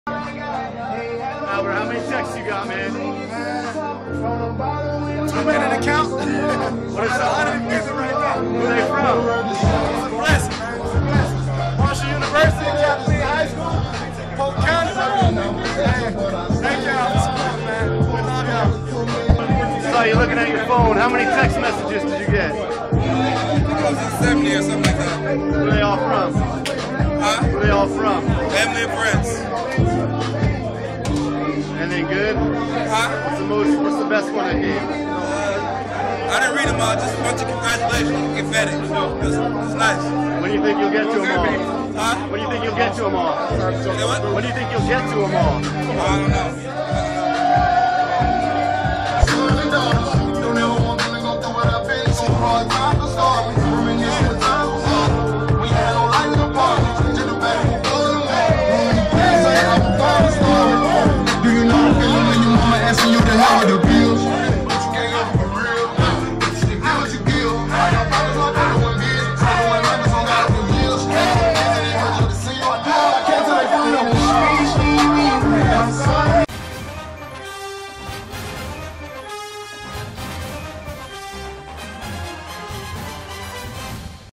What do the Patriots do after winning the Super Bowl? Check their phones! | FOX NFL
What do the Patriots do after winning the Super Bowl? Check their phones!